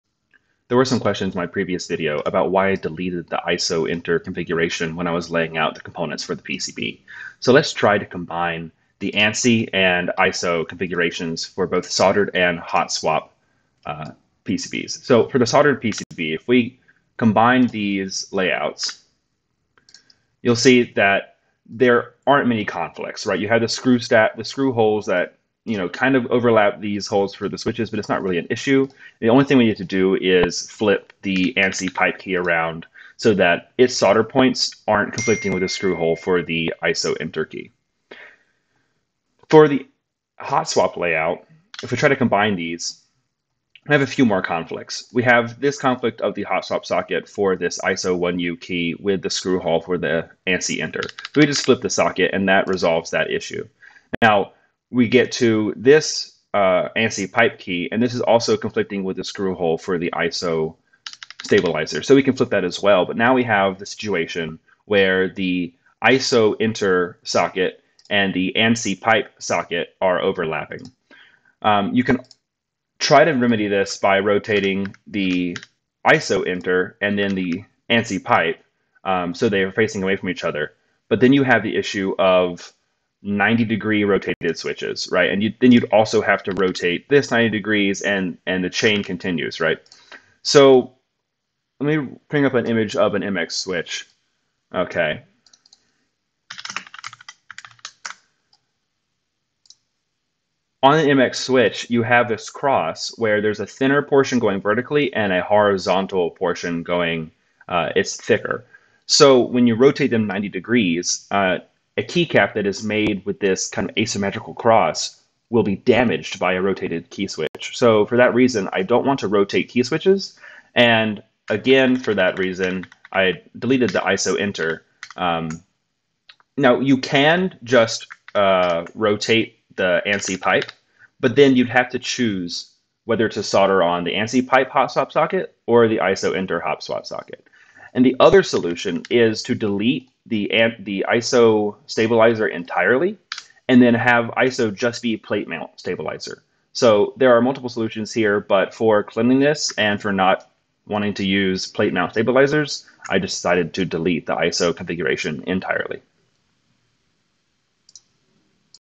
The problem with hotswap and multiple layouts
An addendum for the first part of the TKL PCB Design (Timestamp ), explaining the complications of supporting ANSI and ISO Enter key layouts with hotswap sockets.